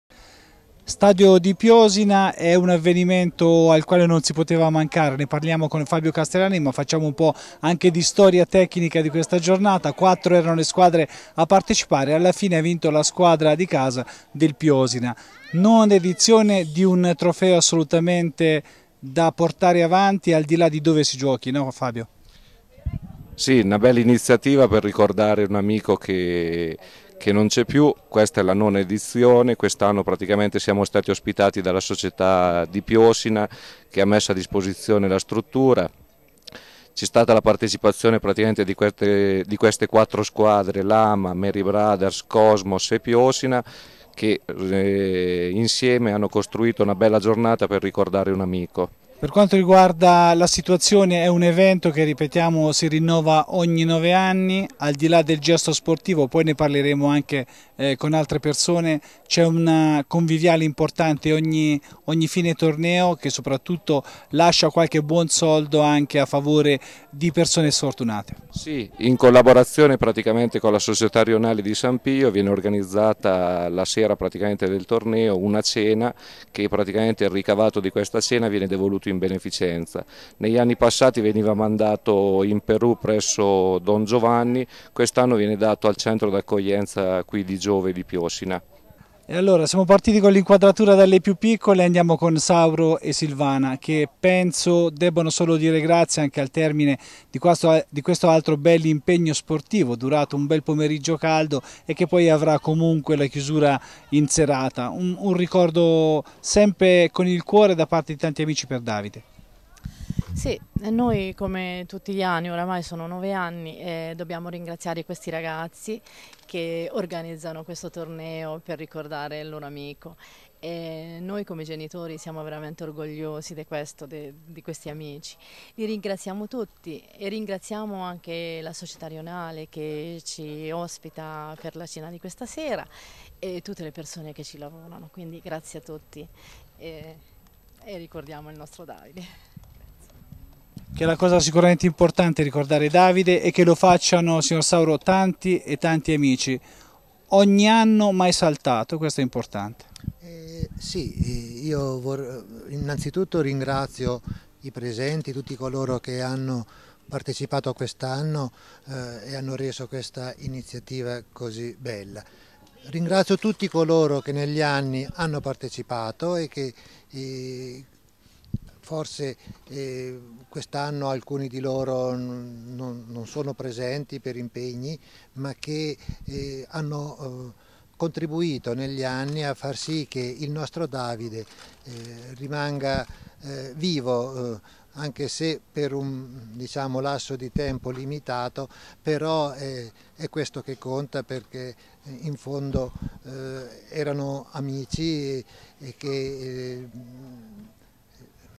9° Memorial Davide Brodi
A Piosina si è ricordato Davide Brodi giocando e divertendosi.

Grande partecipazione e soprattutto emozione allo stadio di Piosina per il 9° Memorial Davide Brodi, il torneo vinto dai padroni di casa ed al quale hanno partecipato le squadre degli Amatori Lama, dei Mary Brothers e dei Cosmos di San Pio ha poi avuto l'appendice nella cena che ha permesso di devolvere l'incasso all'Associazione di Giove. Il servizio